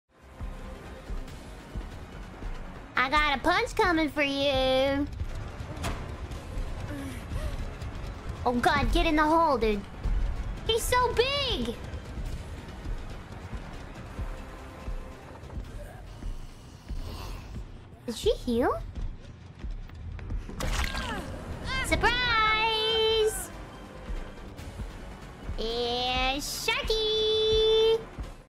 Only click if you wanna hear out of context lewd shark - Gawr Gura
Context: Nemesis is too big to fit the man hole, the hole where you jump down.. in the floor.
Well, he isn't too big but she couldn't fit him in.